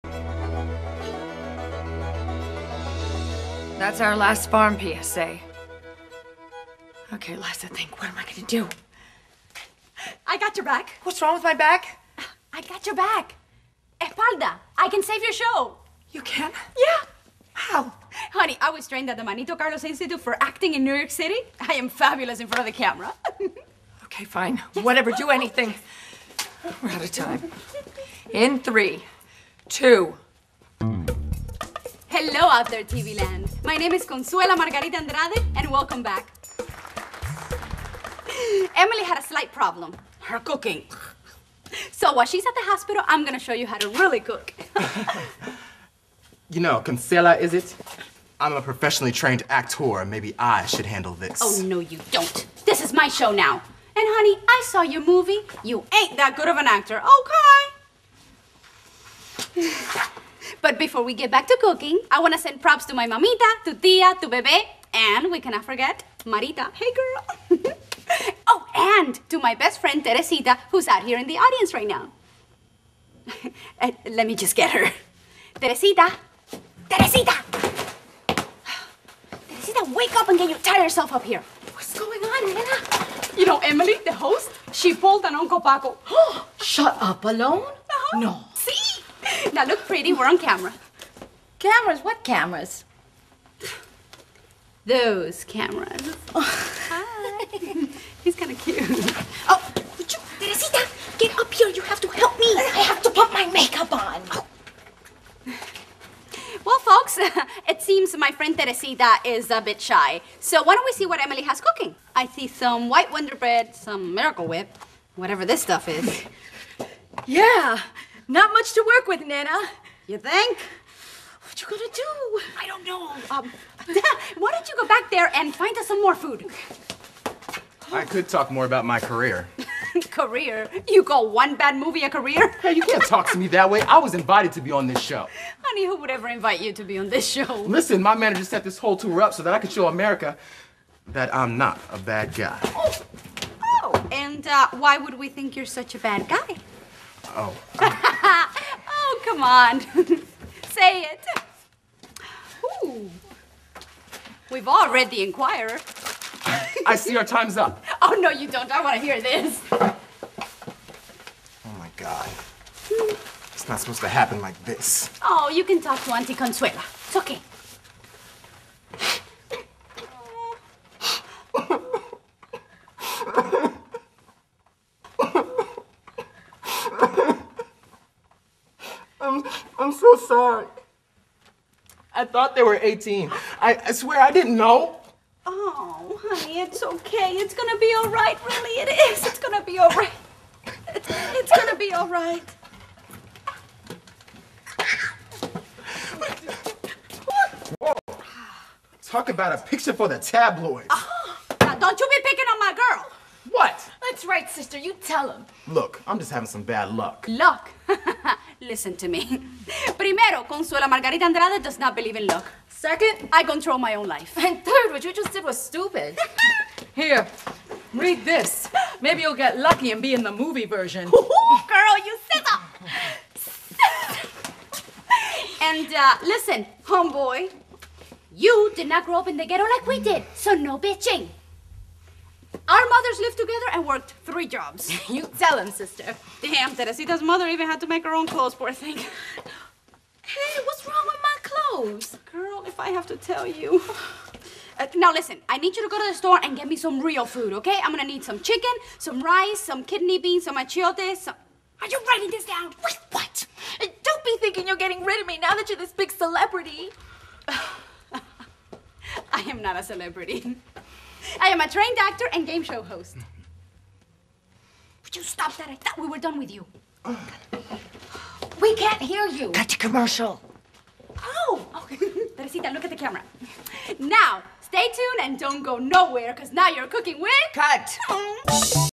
Cookin with Consuela & Teresita (part 4)
a comedy (in 6 parts) about two new yoricans who get stranded in iowa and discover their true talents